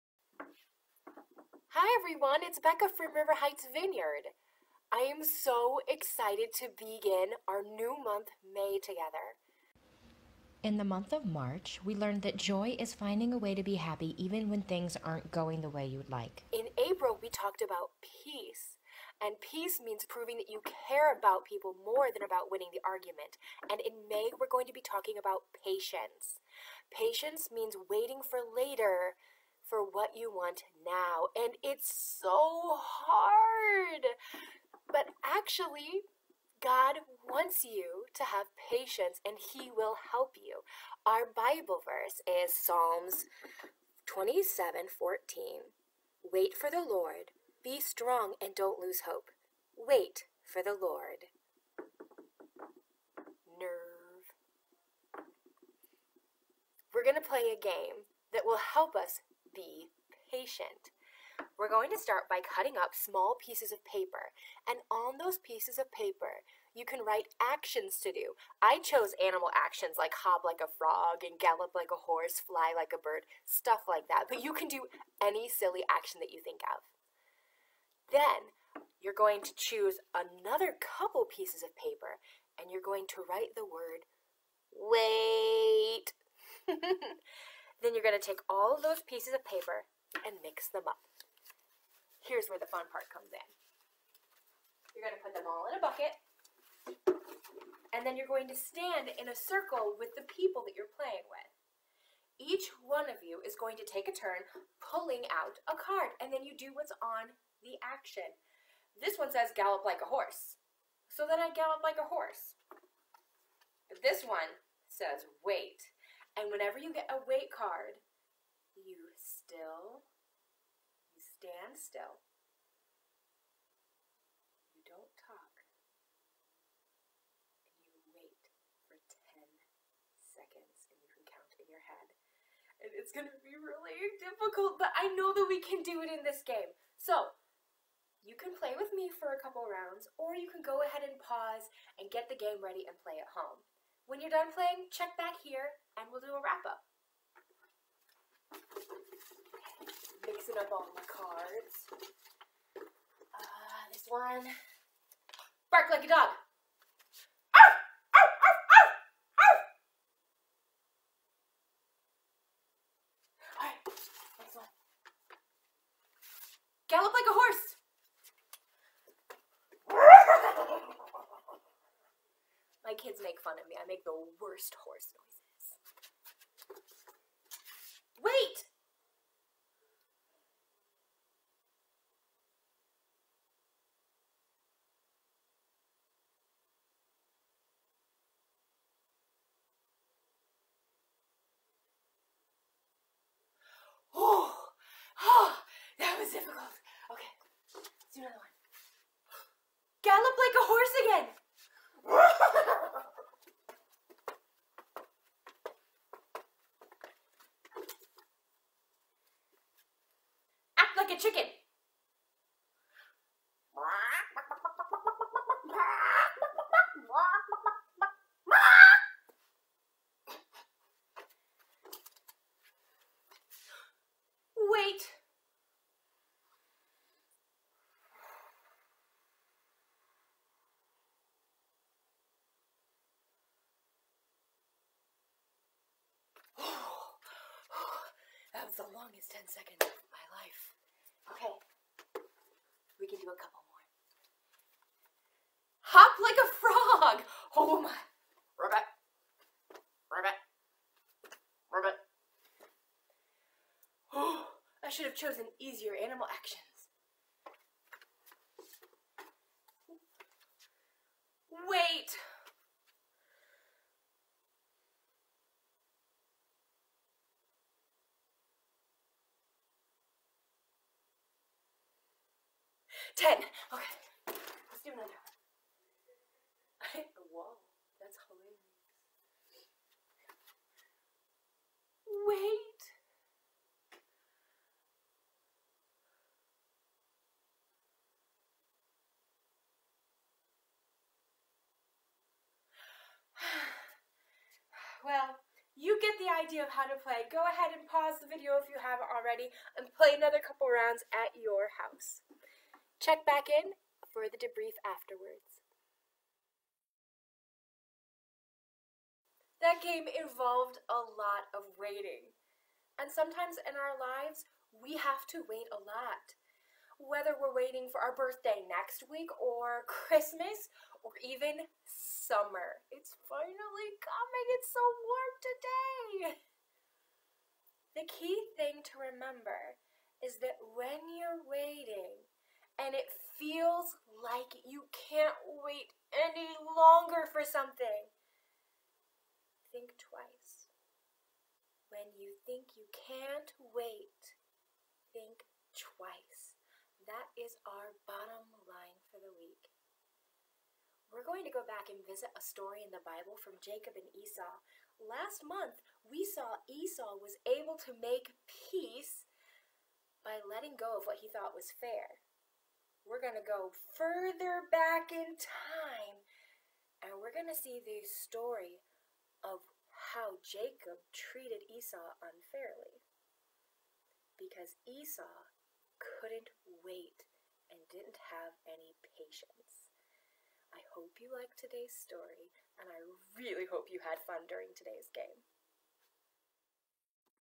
Activity 1 for K-5th Graders - May 3, 2020
K-5th grade activity 1 for the week of Sun, May 3rd.

Needs: strips of paper, writing utensil, bowl